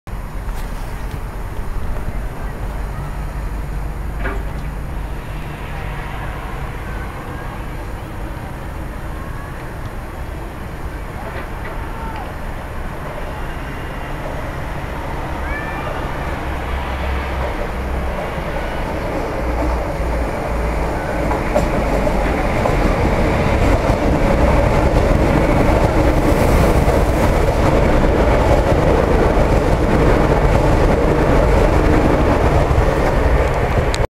Renfe Cercanías/Rodalies de Catalunya - 463.207+465. 237 llegando a Bellvitge
Las UT's 463.207 y 465.237 entran a Bellvitge con un tren de la línea R2.